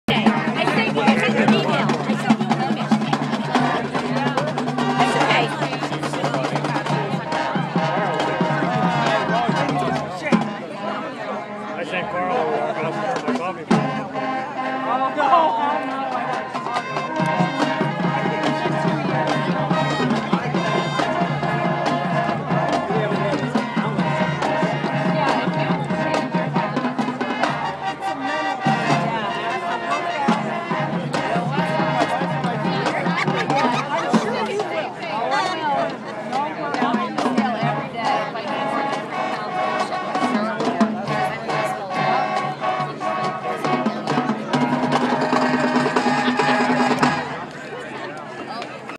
Bloomsburg Band 2012
The Bloomsburg High School Band plays in formation waiting for the Panthers to come on to the field in a home game 2012 football season.